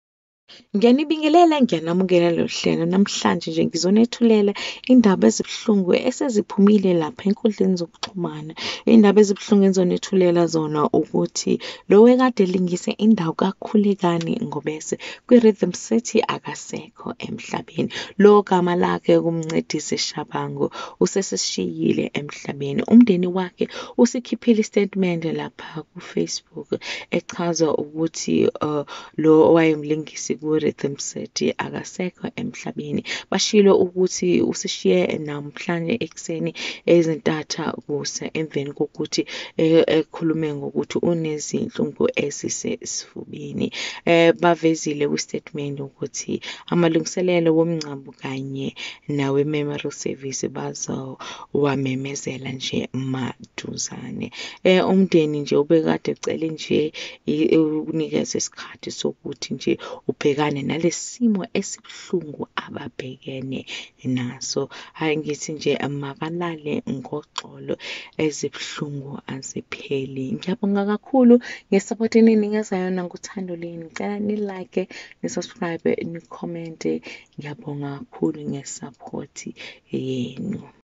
Bad news about Rhythm City actor
Rhythm City actor died at age 53